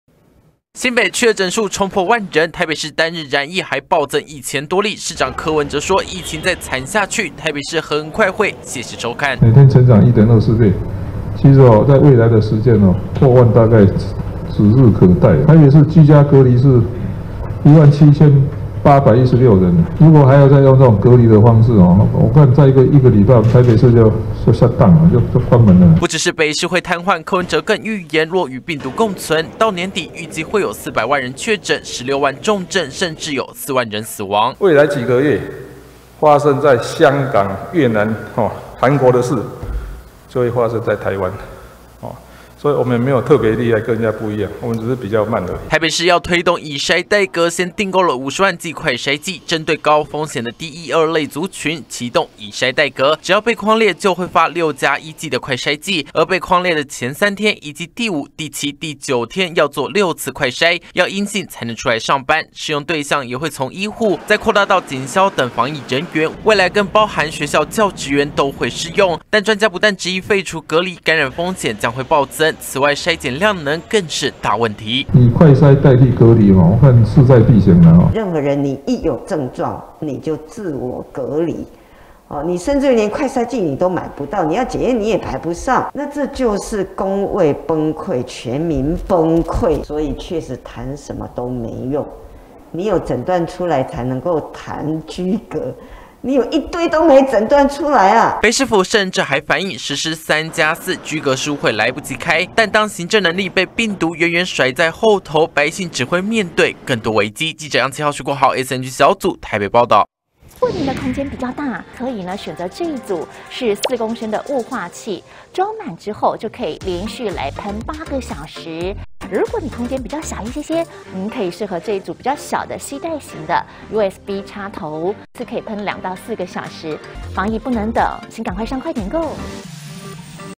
【每日必看】年底4百萬人確診? 柯文哲:台北再隔離"一週將關門"@中天新聞CtiNews @毛球烏托邦MaoUtopia 20220425
雙北疫情嚴峻，台北市長柯文哲預言到年底恐怕會有400萬人確診，16萬人重症甚至有4萬人死亡，北市也喊話要以篩代隔，不然再一個禮拜 北市恐怕就要關門了。